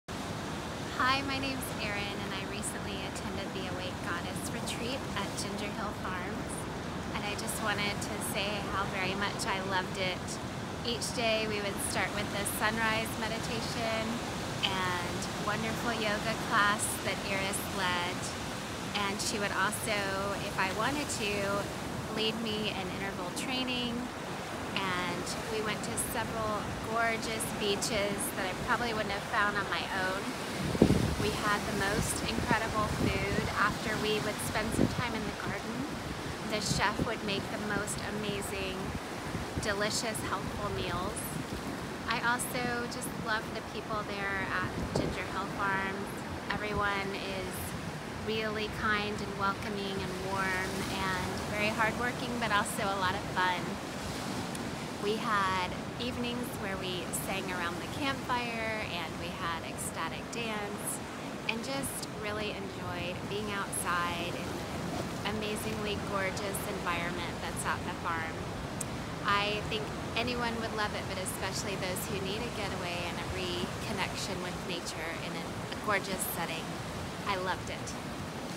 Awake Goddess Retreat - testimonial Eri
She shiny and happy, just ended here Goddess Retreat on Big Island, Hawaii